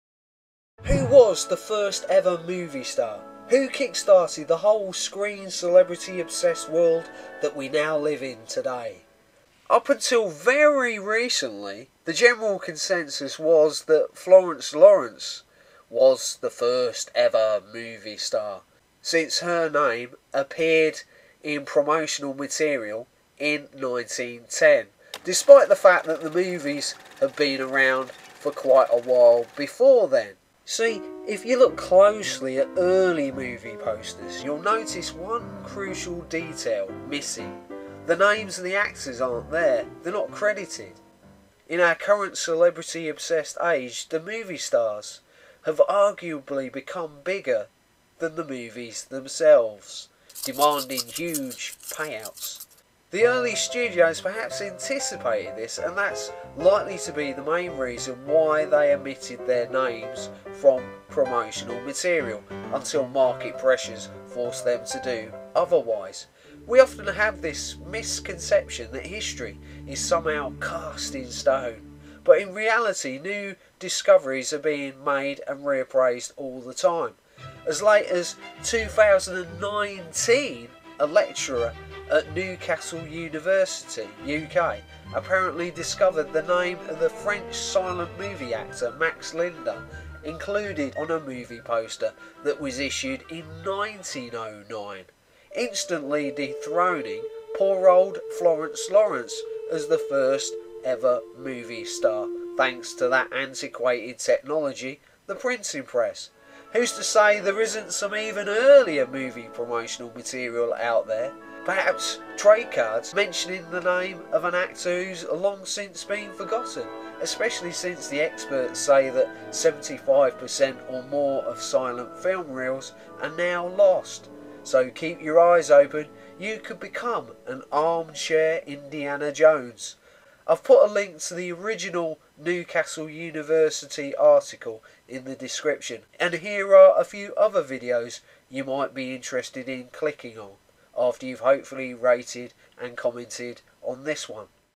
First Movie Star? (earliest) Max Linder
Who was the first movie star ever (earliest)? Recent evidence suggests that it was Max Linder, but some believe it was Florence Lawrence. If you asked the average person in the street they would probably say Charlie Chaplin, since his iconic image is synonymous with the silent era. Why is it so difficult to answer such a seemingly simple question?